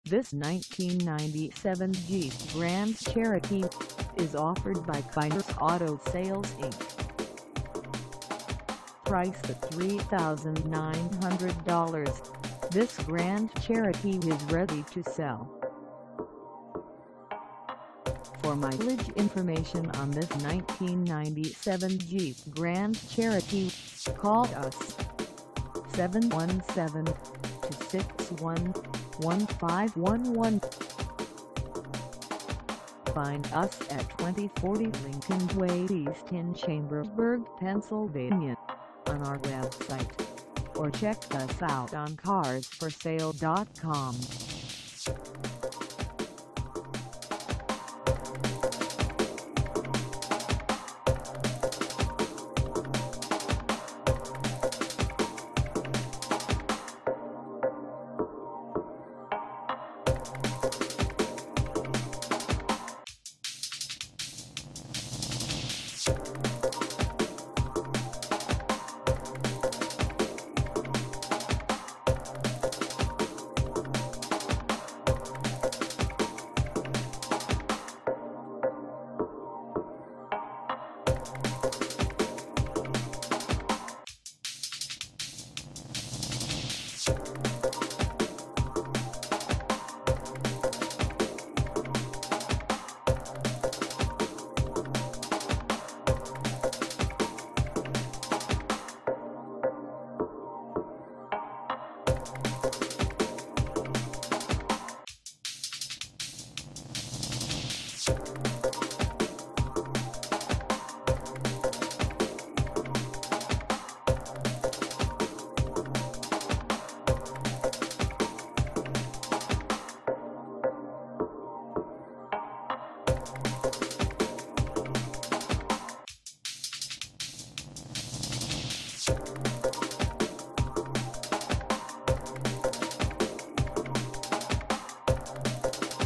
1997 Jeep Grand Cherokee Limited for sale in Chambersburg, P
This 1997 Jeep Grand Cherokee Limited is for sale in Chambersburg, PA 17202 at Kyners Auto Sales INC.
Engine: 8-Cylinder 5.2L V8 16V SPFI OHV
Transmission: 4-Speed Automatic
Color: Silver
Mileage: 0
Contact Kyners Auto Sales INC 2040 Lincoln Way E Chambersburg, PA 17202 to test drive this 1997 Jeep Grand Cherokee Limited today.